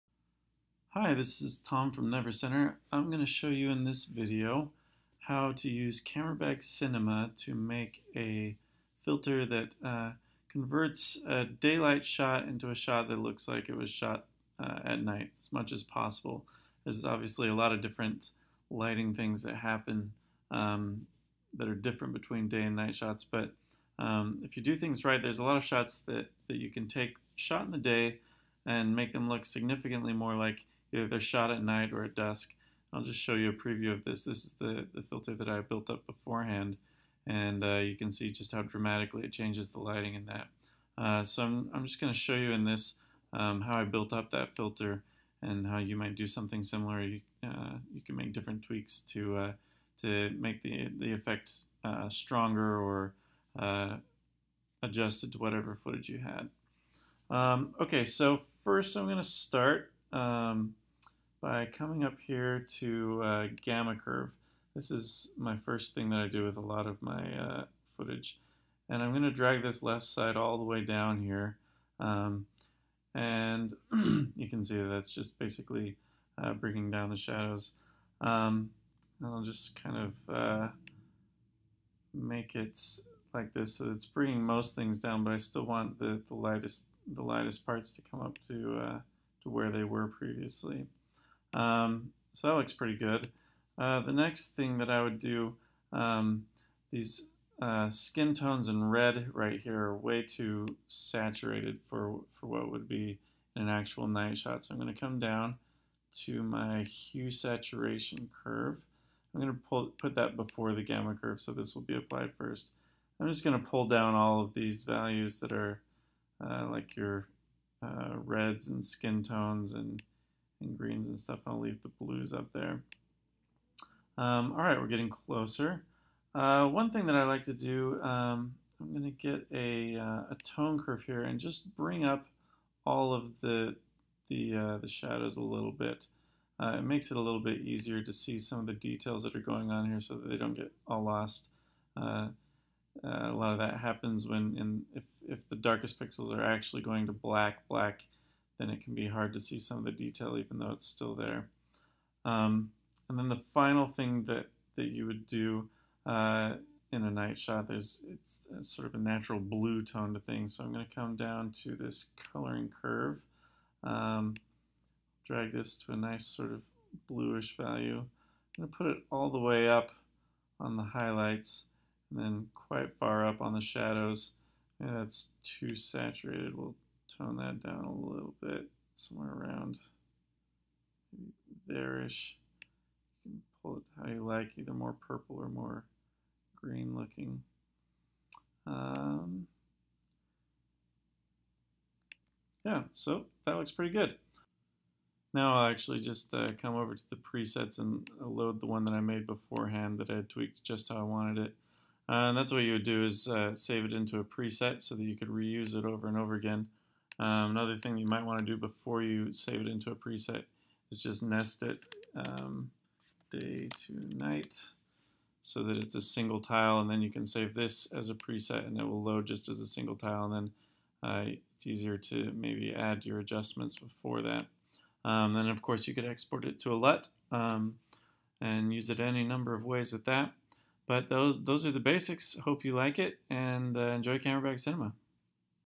CameraBag Cinema - Designing A Day To Night Filter - Free LUT
How to use CameraBag Cinema to make footage shot in the daytime look like it was shot at night. Download the filter (both as a CameraBag .cbf file and as a .cube LUT)